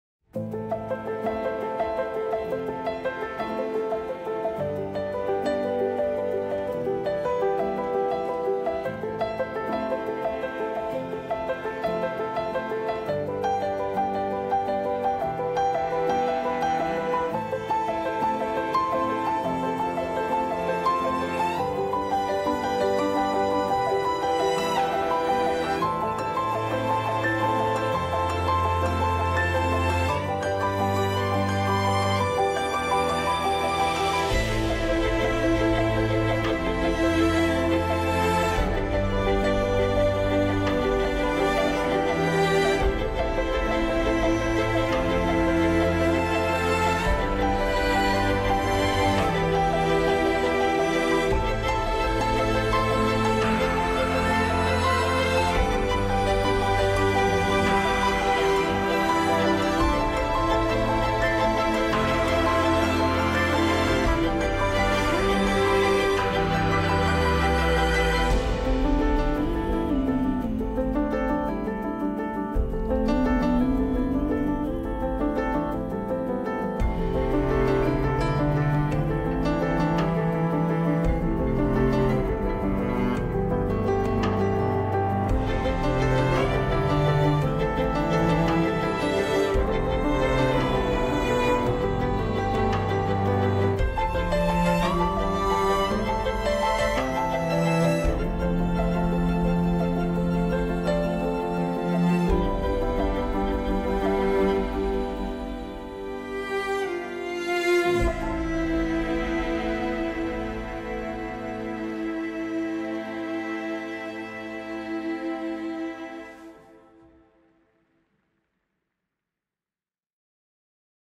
RWM Dive Reel
Shot primarily on Sony A6300. Shots from around the World including Africa, Central America and Asia Pacific.